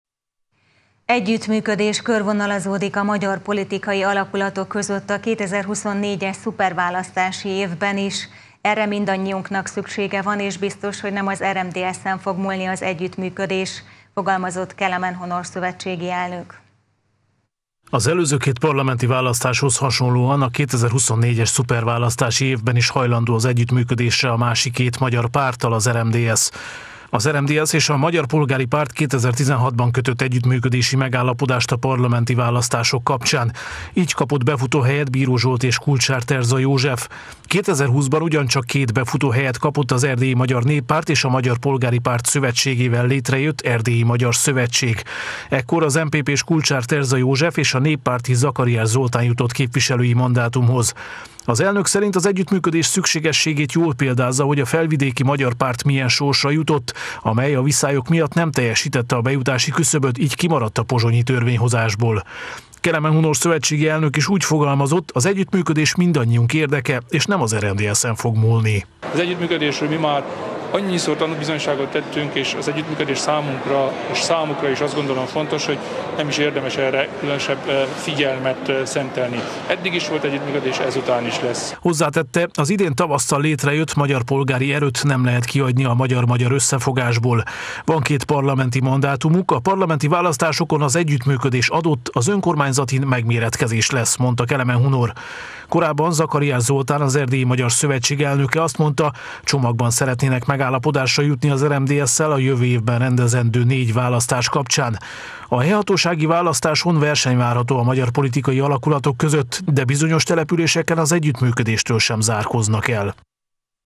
Magyar-magyar együttműködés – Erdélyi Magyar Televízió
Együttműködés körvonalazódik a magyar politikai alakulatok között a 2024-es szuperválasztási évben is. Erre mindannyiunknak szüksége van, és biztos, hogy nem az RMDSZ-en fog múlni az együttműködés – fogalmazott Kelemen Hunor szövetségi elnök.